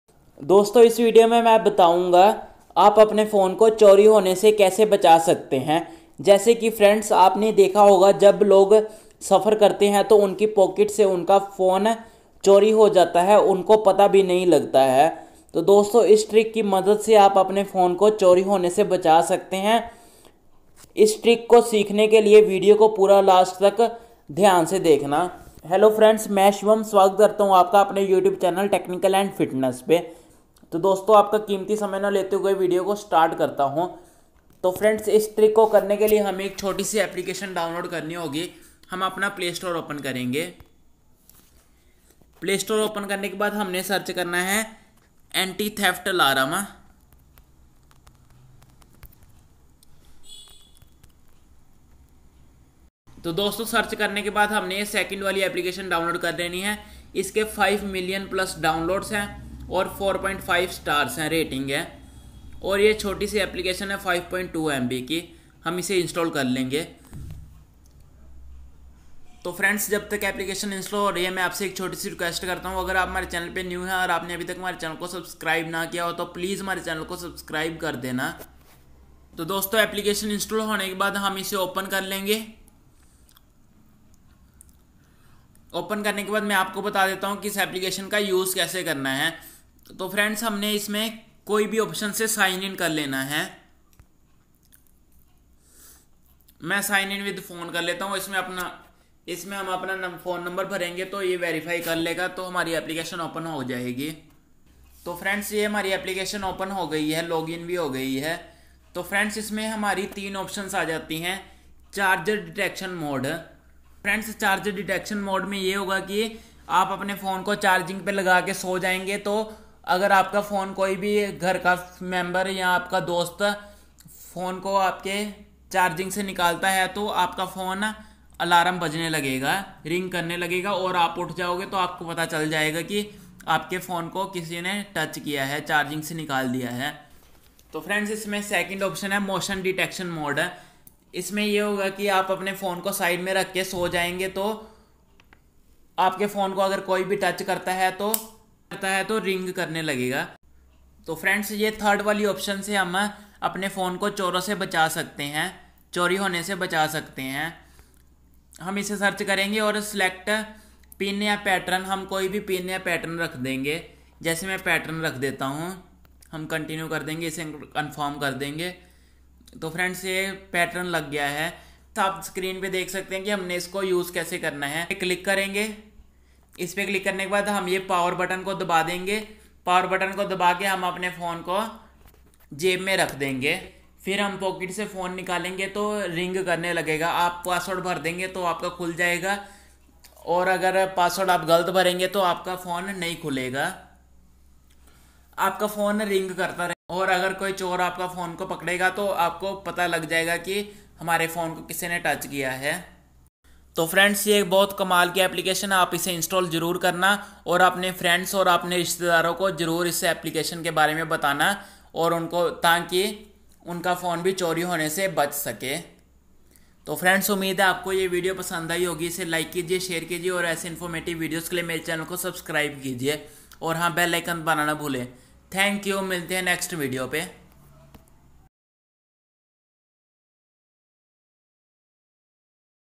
मोबाइल को चोरी होने से कैसे बचाएं।। अपने सभी दोस्तो को भी बता दो🔥🔥
Dosto Iss Video ki madad se aap apne phone ko chori hone se bacha sakte hai. iss video me poora detail me btaya gya h kaise hume kya kya setting karni hai .video ko poora last tak dhyaan se dekhna👉